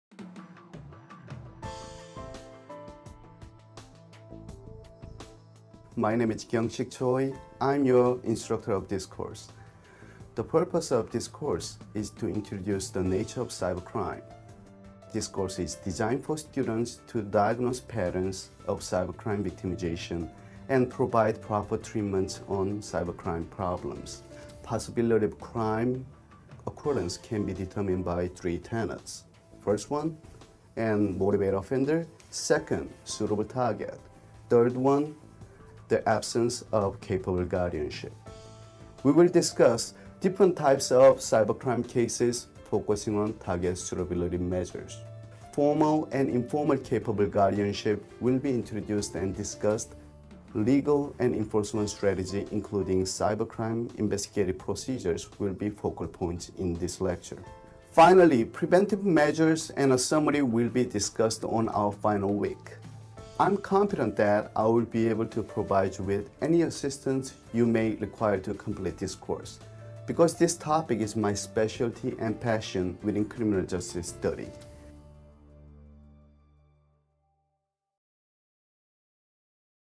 A Brief Look Into - Special Topics: Cybercrime
In this course, Prof. Kyong-Shick Choi will introduce students to the nature and types of cybercrime.

Offered as part of the Master of Criminal Justice Program at Boston University's Metropolitan College.
(CJ 610)

Transcript:
Kyung-Shick Choi: "My name is Kyung-Shick Choi. I'm your instructor of this course.The purpose of this course is to introduce the nature of cybercrime. This course is to designed for students to diagnose patterns of cybercrime victimization and provide proper treatment of cybercrime problems. Possibility of crime accordance can be determined by three tenets. The first one: motivated offender, second: suitable target, third one: the absence of capable guardianship. We will discuss different types of cybercrime cases. For example, target durability measures. Formal and informal guardianship will be introduced and discussed. Legal and enforcement strategies including cybercrime investigated procedures will be focal points in this lecture. Finally, prevented measures and a summary will discussed on our final week. I'm competent that I will be able to provide you with any assistance you may require to complete this course. Because this topic is my speciality and passion within criminal justice theory."